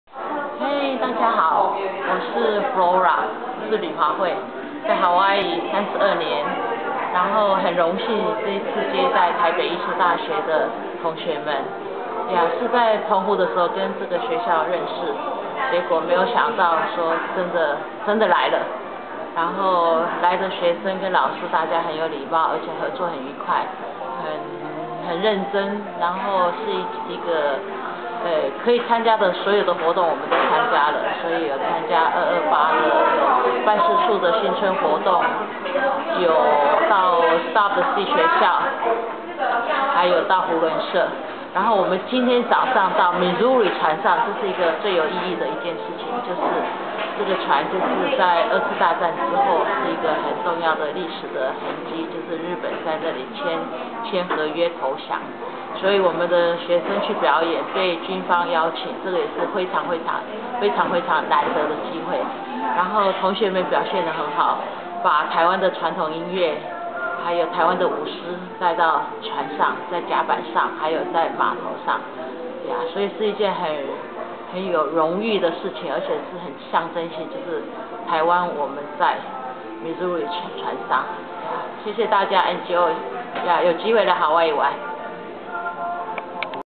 北藝大傳音系於夏威夷專訪Flora Lu女士
Flora Lu女士為活躍於台美各地的僑胞，是邀請北藝大赴夏威夷演出的關鍵人物。詳情請搜尋「關渡音樂學刊」「夥努嚕嚕嚕陣頭」